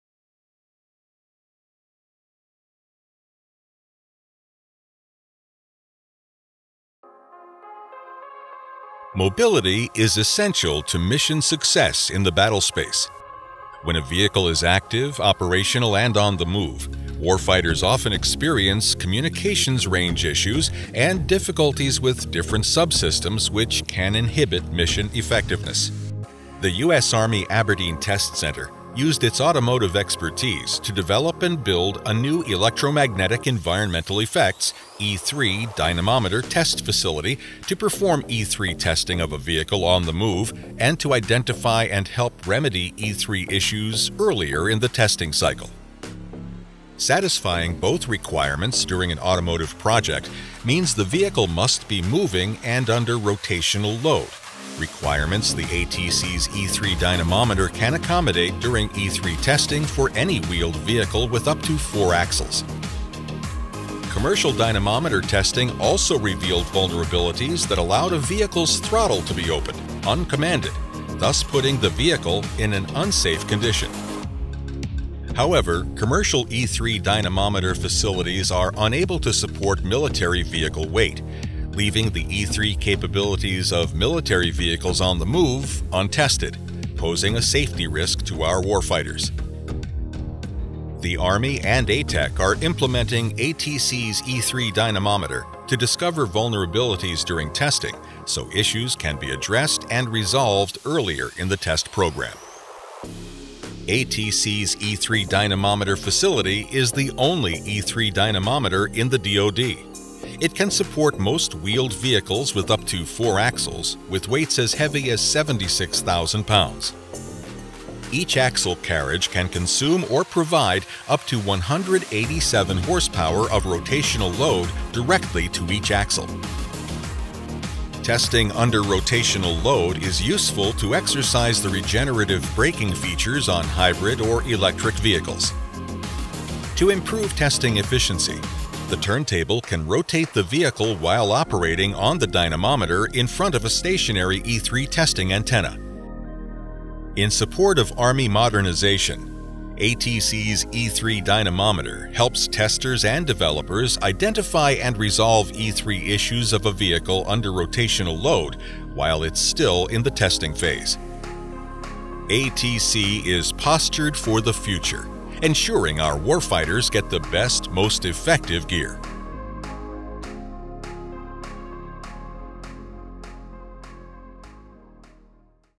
ATC's E3 Dynamometer
The U.S. Army Aberdeen Test Center's Electromagnetic Environmental Effects (E3) chamber is the only E3 Dynamometer in the DoD capable of supporting the weight of wheeled military vehicles (and meeting the testing requirements in MIL-STD-461, MIL-STD-464 and ATPD-2407). We test to support Army transformation and to ensure our U.S. warfighters are equipped and ready for mission success, anywhere in the world.